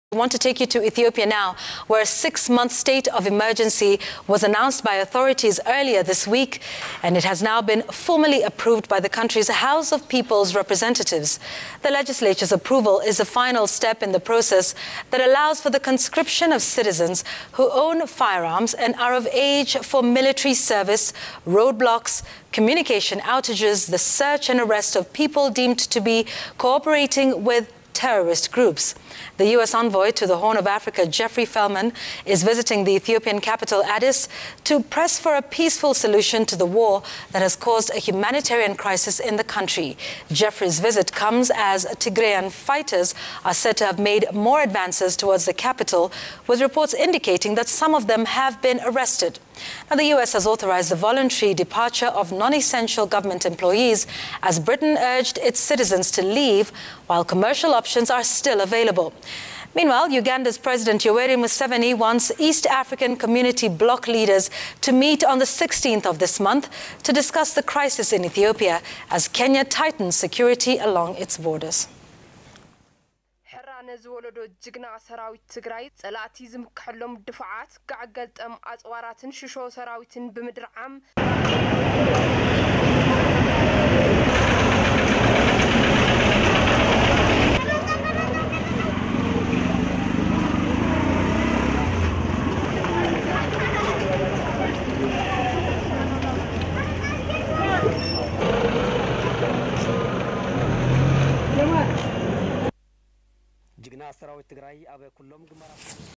Ethiopia’s house of representatives approves state of emergency
State of Emergency allows conscription of armed civilians
US envoy Jeffrey Felman in Ethiopia to seek peaceful resolution